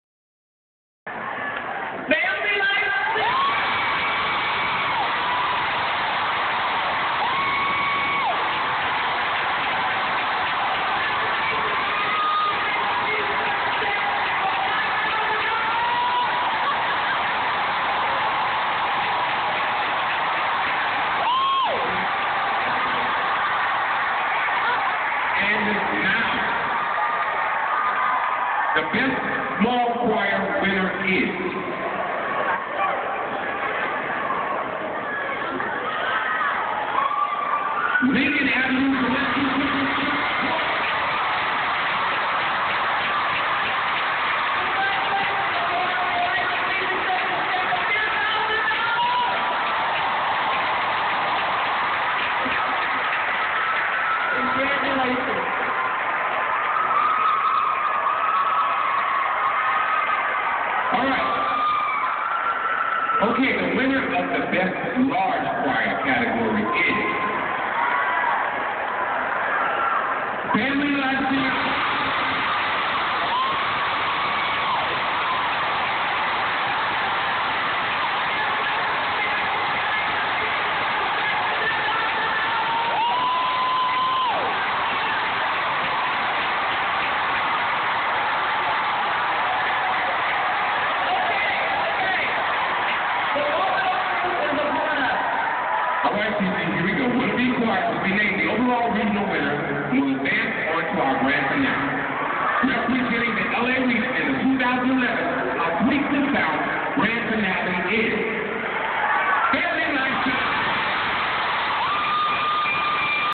FLC Winning @ How Sweet The Sound 2011!
Family Life Center Sanctuary Choir @ How Sweet The Sound, Staples Center, 10/1/11